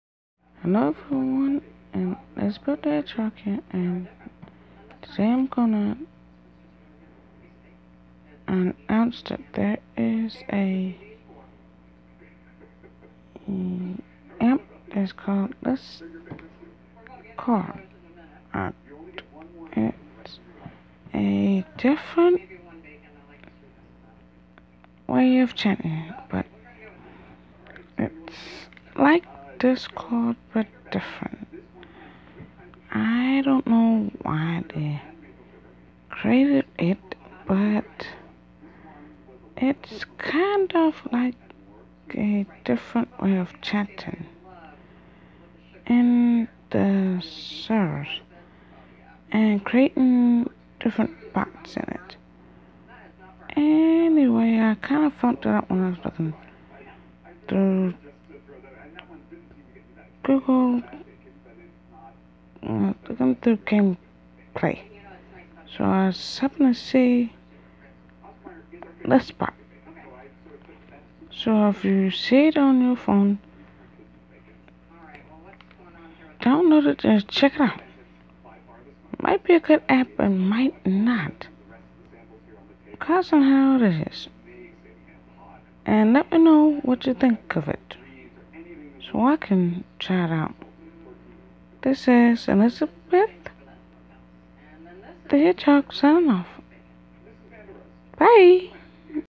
A New App Is Out Called Listcord Check It Out
It is a new version of discord not sure if it good or not but I haven't download it to find out & enjoy the video bye & stay safe 😀.